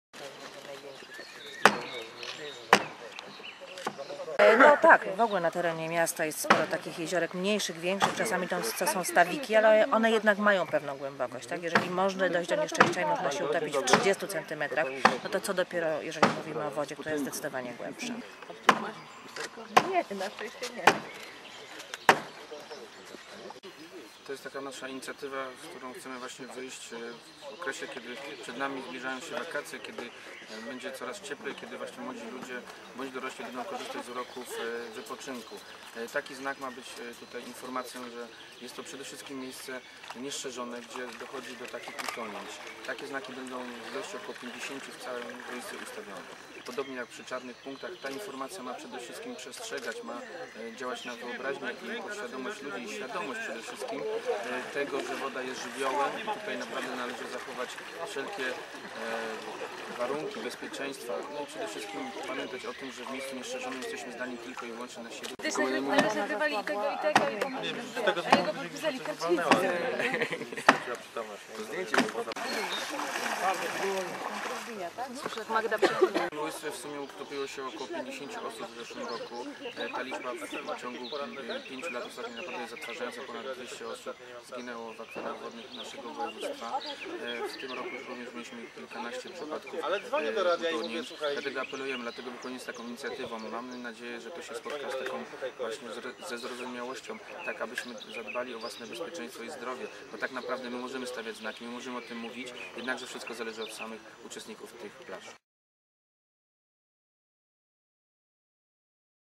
Policyjne tablice ostrzegą przed utonięciem
Mają ostrzegać przed brawurową kąpielą na niestrzeżonych plażach. Pierwszą z pięćdziesięciu tablic z napisem "utonięcia" wbito w dno jeziora Goplana w Szczecinie.